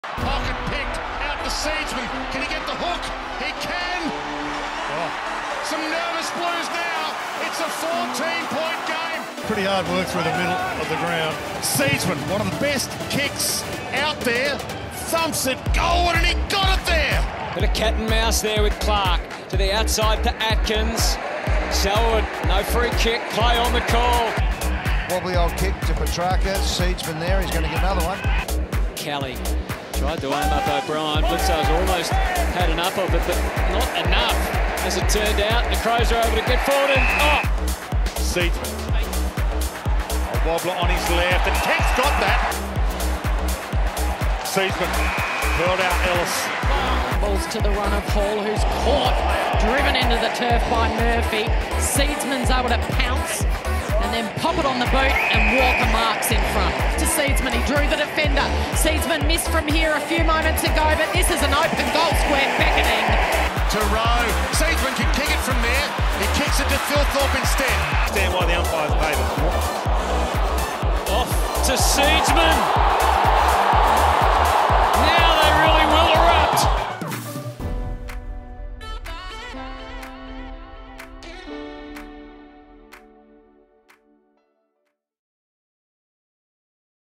Paul Seedsman Re-signs
Speedy wingman Paul Seedsman has re-signed with the Crows.